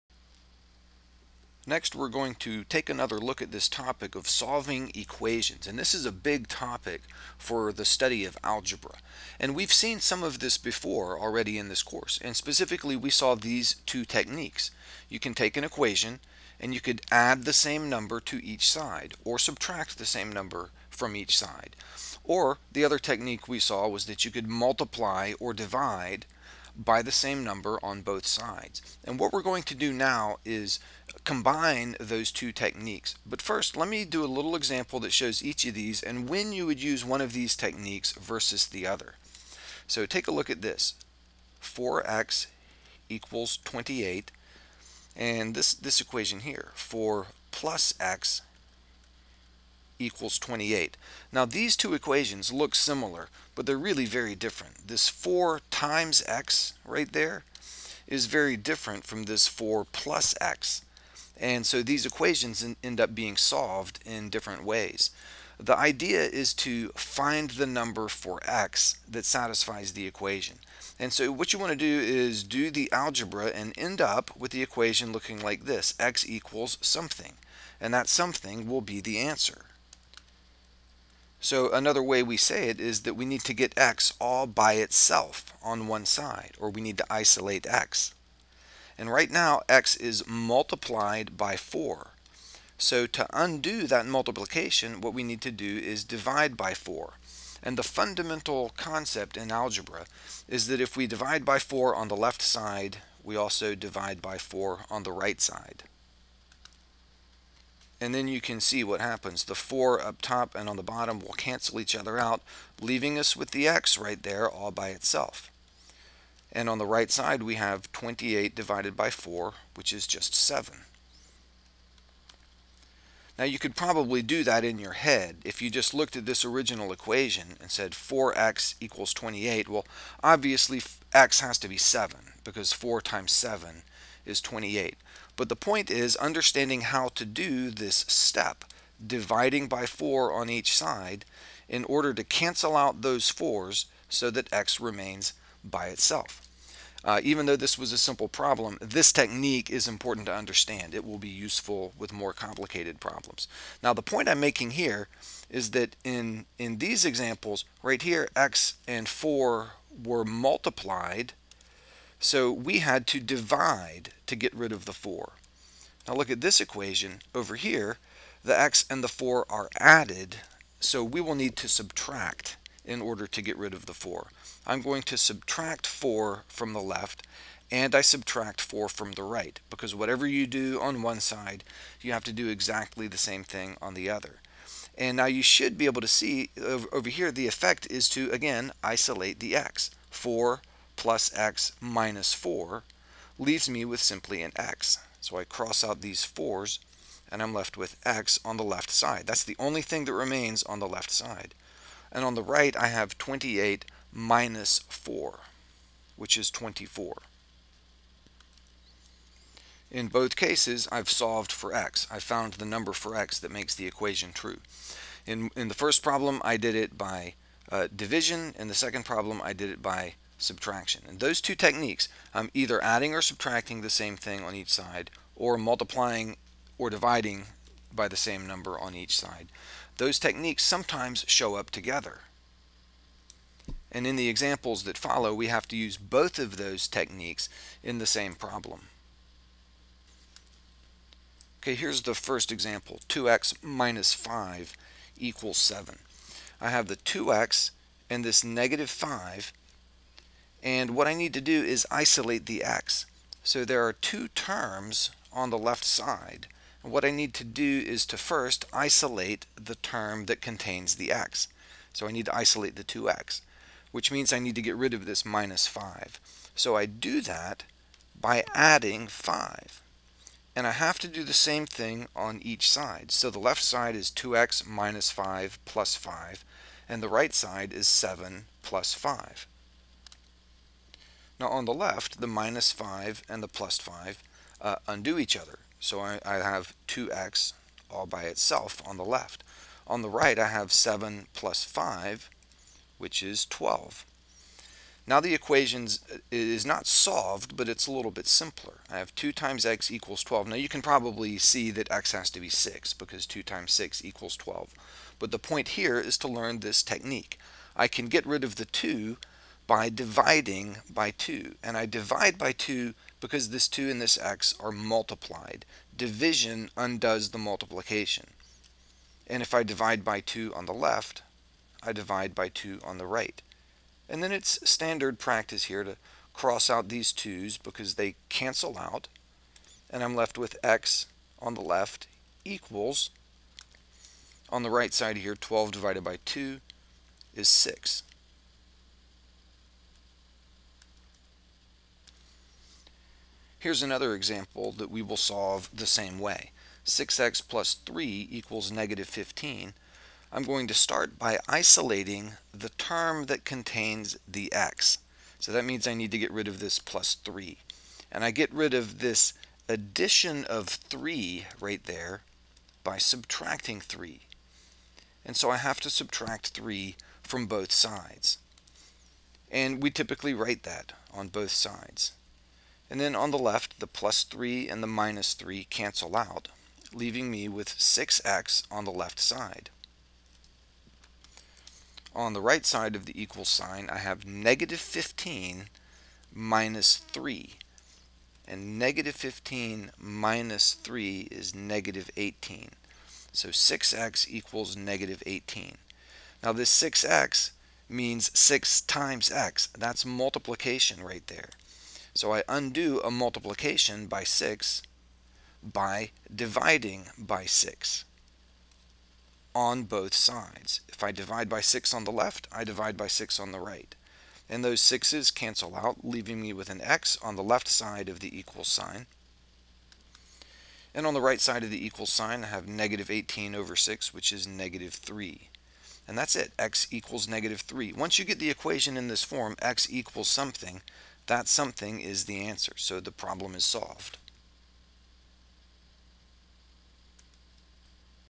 Prealgebra 3.06a - Linear Equations in One Variable
Solving linear equations.  Adding and subtraction the same quantity on both sides.  Multiplying and dividing by the same quantity on both sides.  The basic concepts of Algebra are demonstrated through some introductory examples.